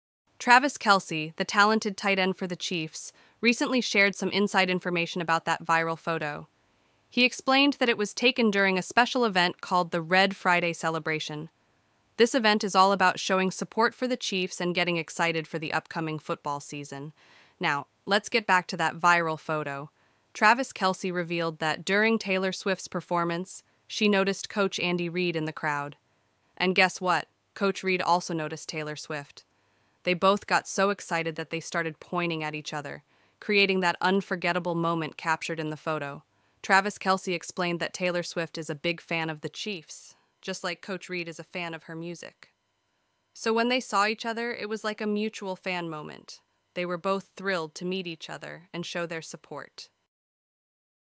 Travis Kelce explains viral photo of Taylor Swift and Chiefs coach Andy, 31th January 2024
Travis Kelce explains viral photo of Taylor Swift and Chiefs coach Andy
"Travis Kelce, the talented tight end for the Chiefs, recently shared some inside information about that viral photo. He explained that it was taken during a special event called the "Red Friday" celebration. This event is all about showing support for the Chiefs and getting excited for the upcoming football season. Now, let's get back to that viral photo. Travis Kelce revealed that during Taylor Swift's performance, she noticed Coach Andy Reid in the crowd. And guess what? Coach Reid also noticed Taylor Swift! They both got so excited that they started pointing at each other, creating that unforgettable moment captured in the photo.Travis Kelce explained that Taylor Swift is a big fan of the Chiefs, just like Coach Reid is a fan of her music. So when they saw each other, it was like a mutual fan moment. They were both thrilled to meet each other and show their support".,